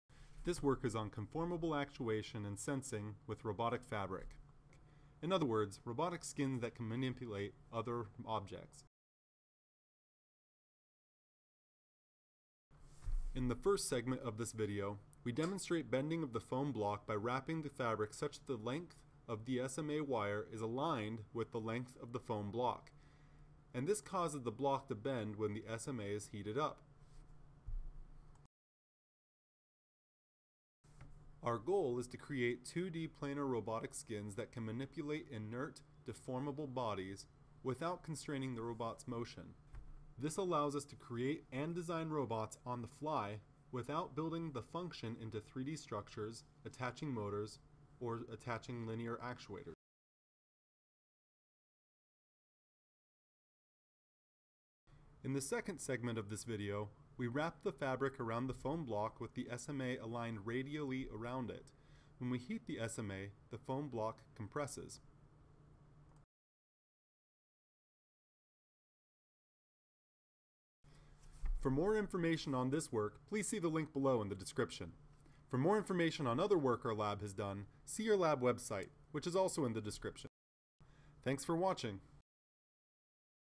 Active Robotic Skin
This video shows an initial active skin that can deform foam blocks.  This is a first step toward robot skins that can wrap around soft things and be used to quickly prototype robots.